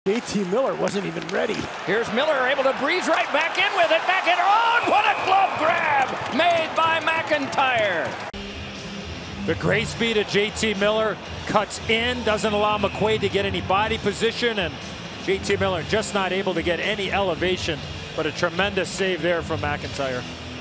Bruins’ McIntyre makes glove save
Watch as Boston Bruins goaltender Zane McIntyre makes a glove save against the New York Rangers.